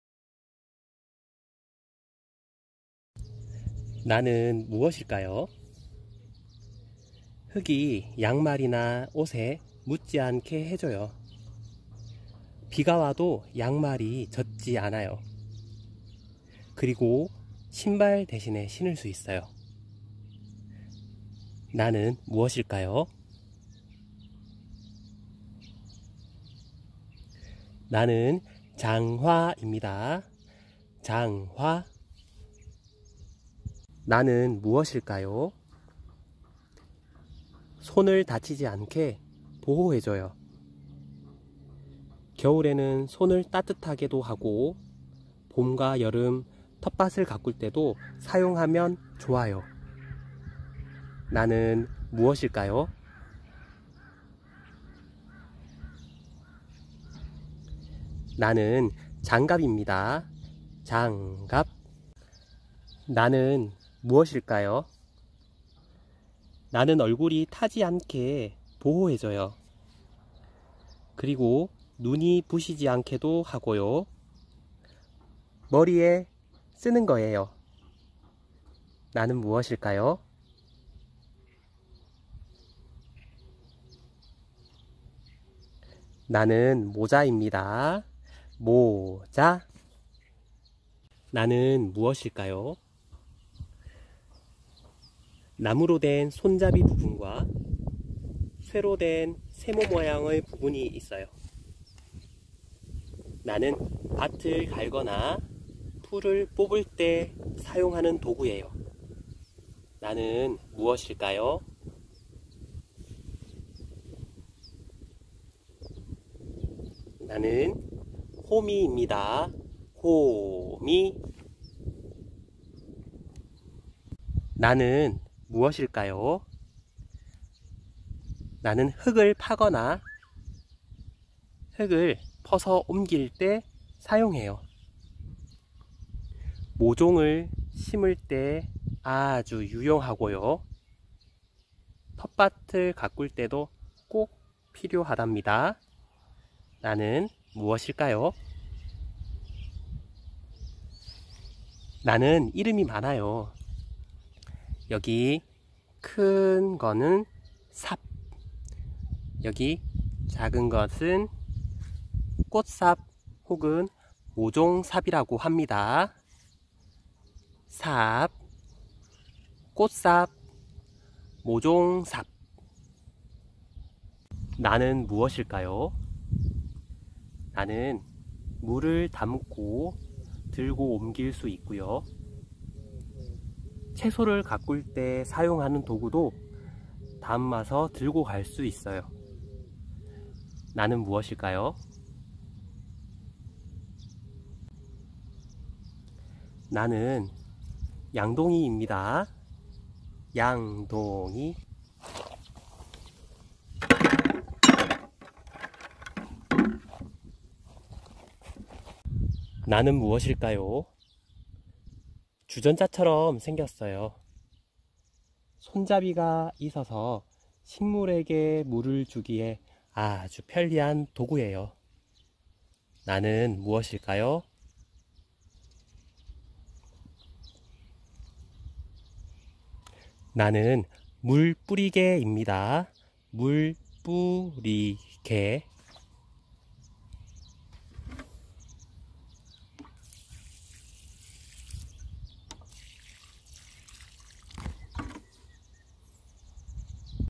텃밭 수업 | 텃밭 도구 퀴즈 | 텃밭에 필요한 물건 | 세 고개 퀴즈 | 특수학교 온라인 수업 자료 | 특수교사 순해샘 | 기본교육과정 통합교과 봄
■ 관련단원

기본교육과정 통합교과 (나)-1
2단원 우리 가족 행복 텃밭
3~4차시 함께 준비해요

퀴즈 정답: 장화, 모자, 장갑, 양동이, 호미, 모종삽, 물뿌리개

stop & play 하며 학생이 생각하고 표현할 수 있는 기회를 많이 주면 좋을거 같습니다.